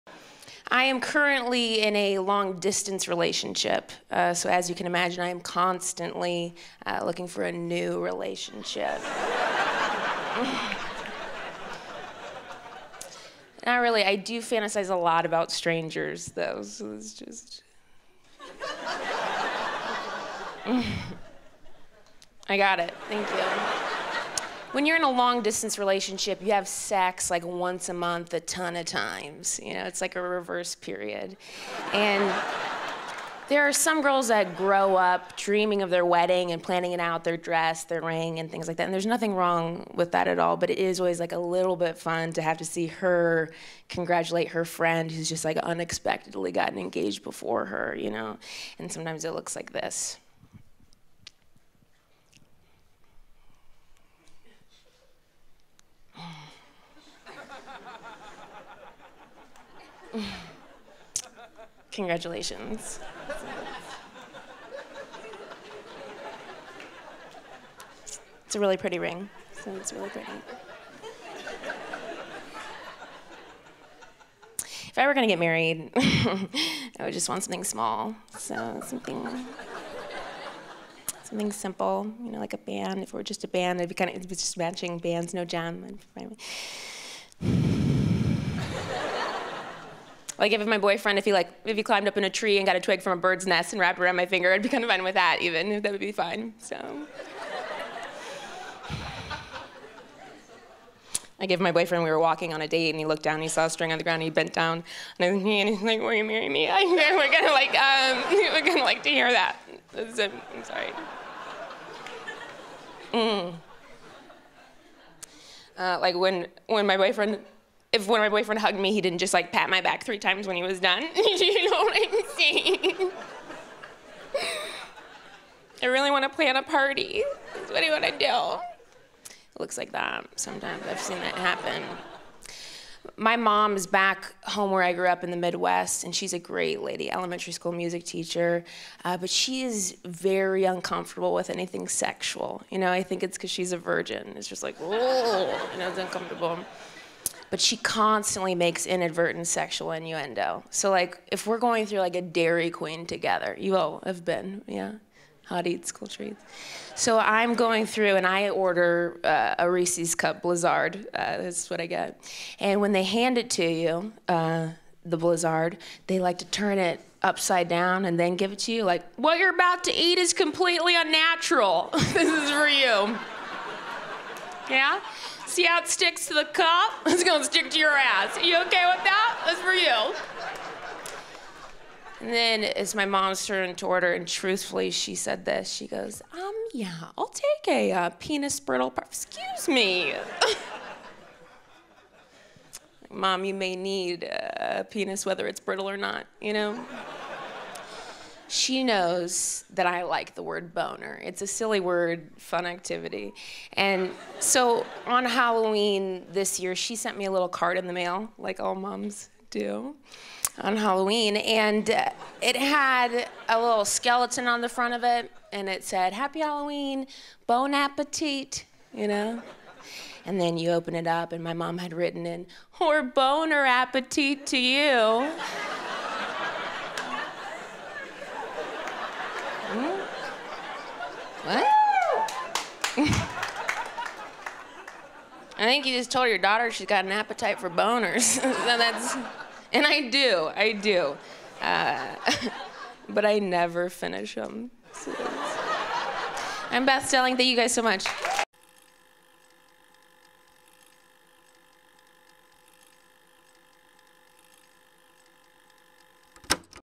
Beth Stelling - Mom's Accidental Sexual Innuendos
BethStelling on weddings and her mom's accidental sexual innuendos.

BethStellingStandUp from the JustForLaughs Festival in 2013.